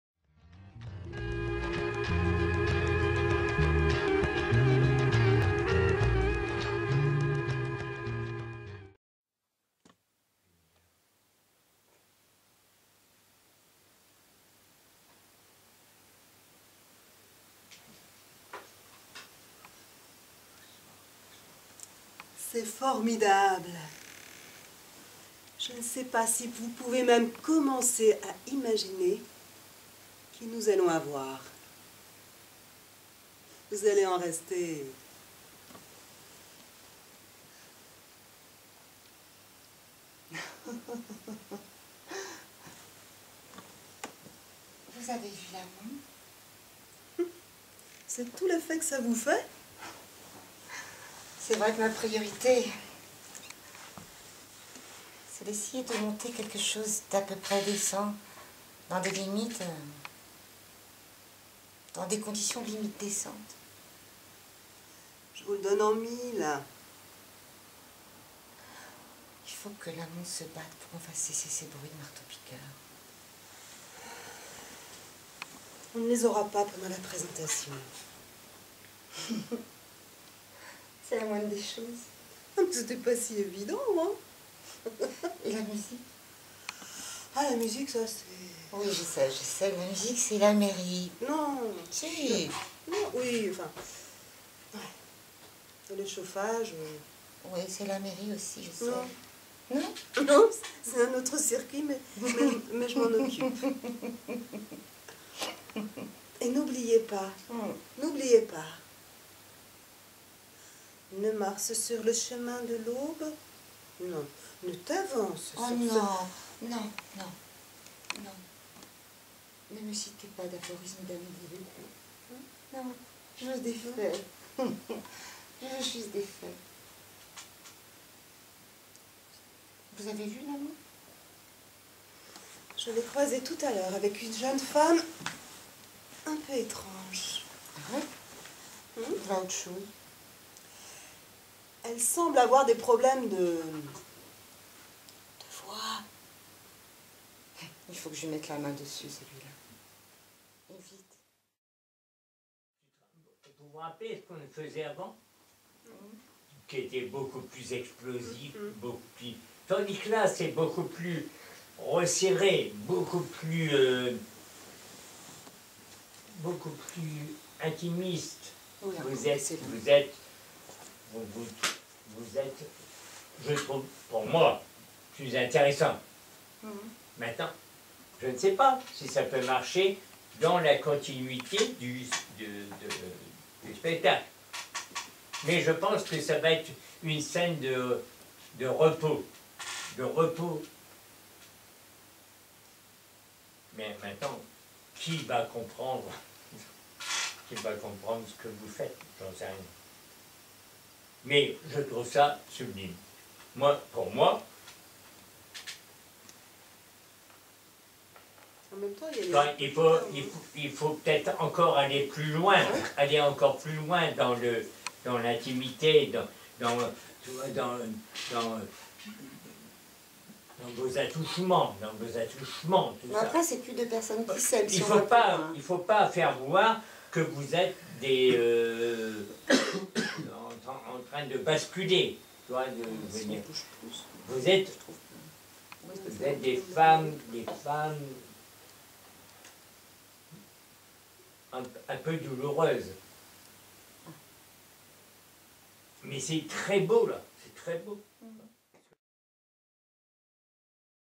Noir Lumière - Webisode Répétitions 03
Aude et Myriam répète un dialogue de "Nefs Et Naufrages" dans un jeu jamais encore exploité pour ce texte.